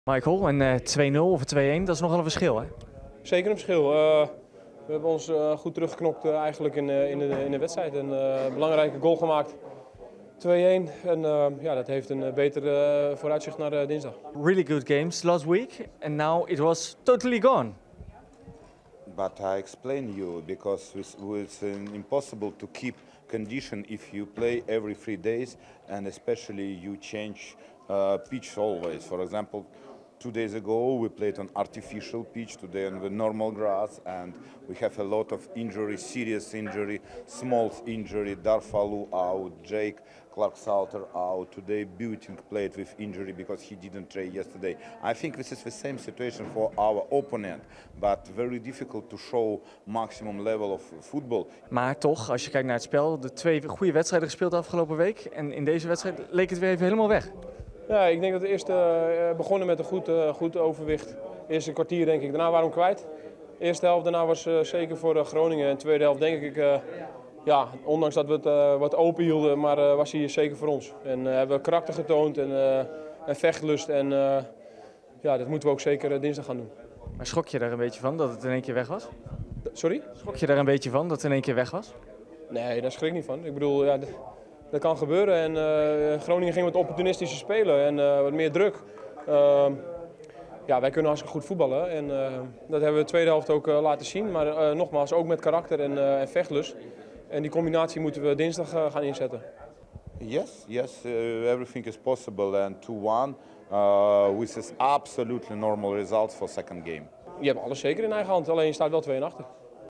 Zwak Vitesse mag na nederlaag in Groningen nog hopen op finale play-offs
GRONINGEN - Vitesse heeft de eerste wedstrijd in de play-offs om Europees voetbal verdiend verloren. In en tegen Groningen werd het zaterdagavond 2-1.